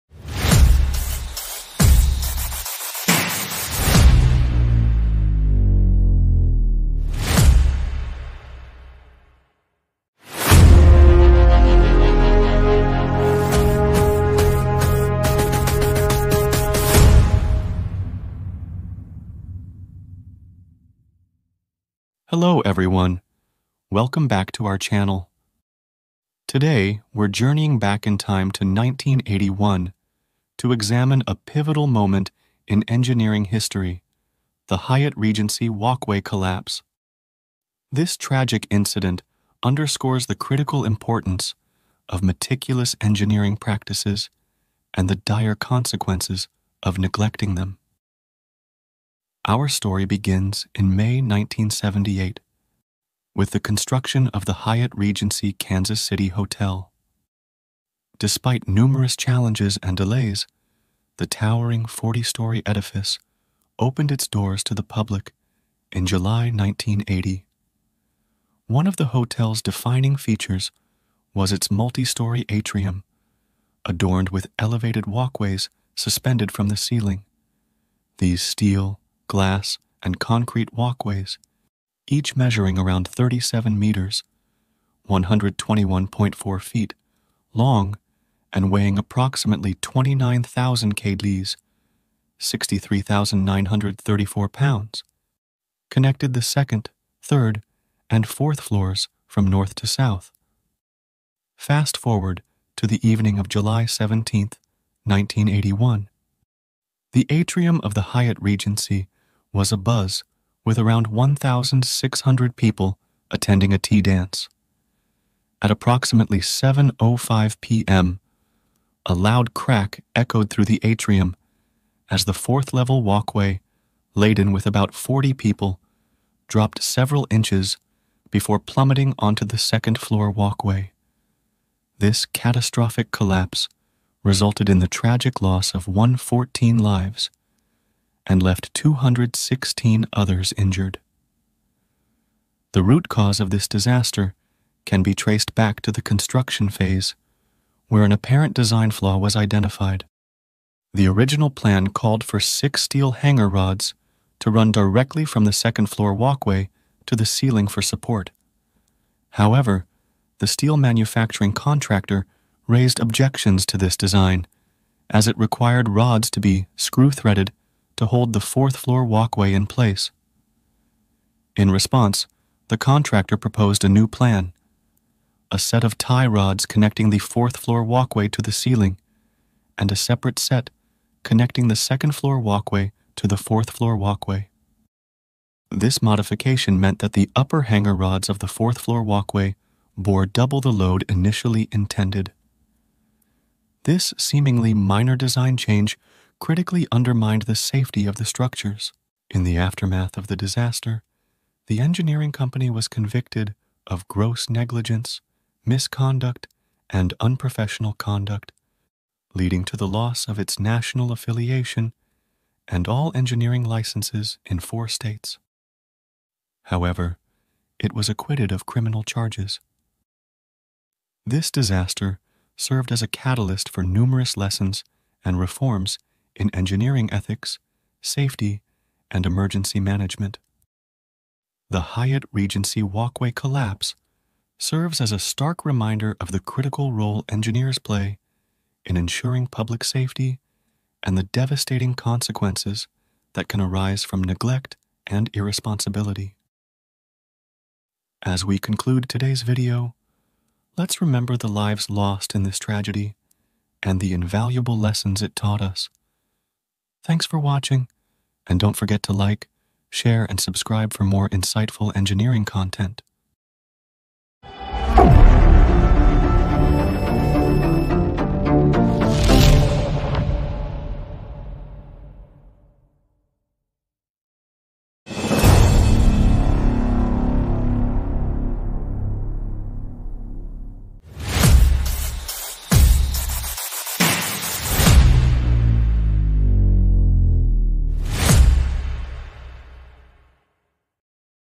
Hyatt Regency Walkway Collapse: A Case Study in Engineering Failure
Join us as we delve into a significant event in engineering history - the Hyatt Regency walkway collapse of 1981. This video explores the circumstances leading up to this tragic incident, the critical engineering errors that contributed to it, and the profound impact it had on engineering practices. We’ll discuss the construction of the Hyatt Regency Kansas City Hotel, the design flaws in its walkways, and the catastrophic collapse that claimed 114 lives. We’ll also examine the aftermath of the disaster, including the legal consequences for the engineering company involved and the subsequent reforms in engineering ethics, safety, and emergency management. This video serves as a stark reminder of the vital role engineers play in ensuring public safety and the devastating consequences of neglecting this responsibility.